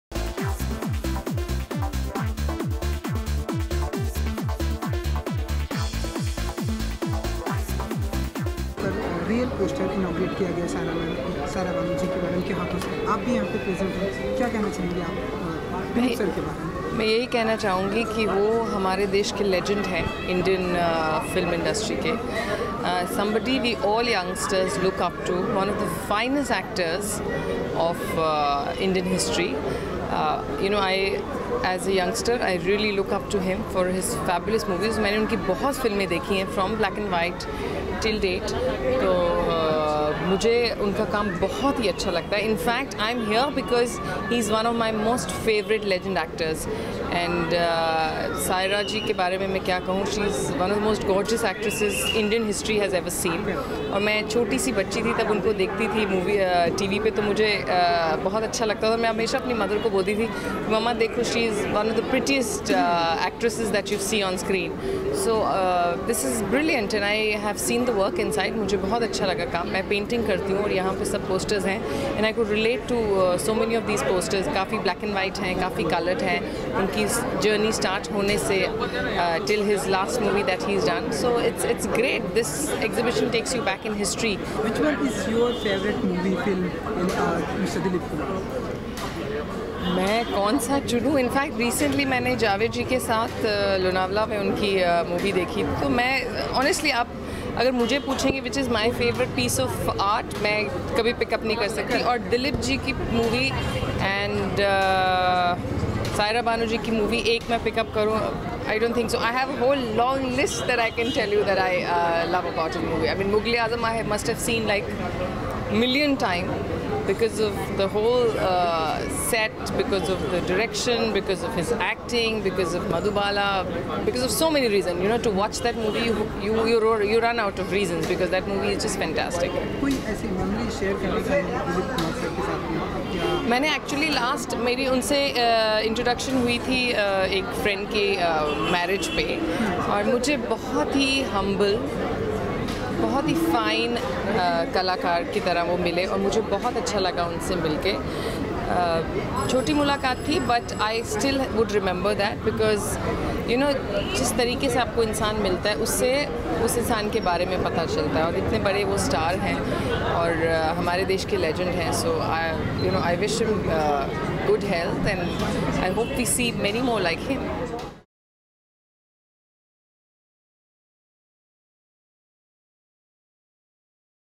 Rouble Nagi at the Two Thespians - Dilip Kumar and Marlon Brando festival at Osianama in Liberty
Rouble Nagi at the Two Thespians - Dilip Kumar and Marlon Brando festival at Osianama in Liberty Cinema